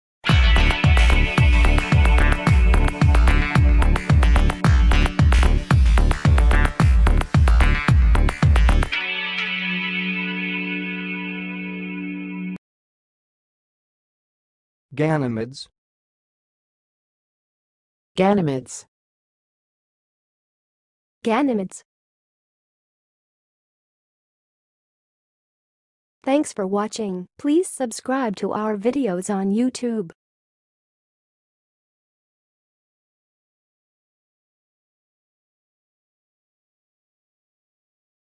How to say "ganymedes"! (High Quality Voices)
Watch in this video how to say and pronounce "ganymedes"!